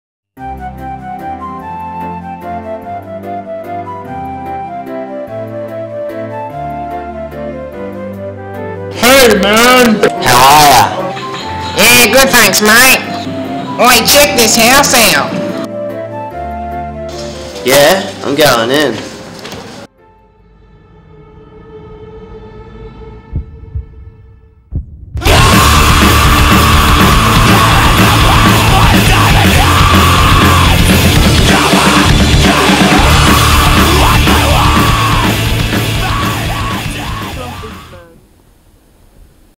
The Crazy Worm
Year 9 or 10 Photography project using stills to make a movie..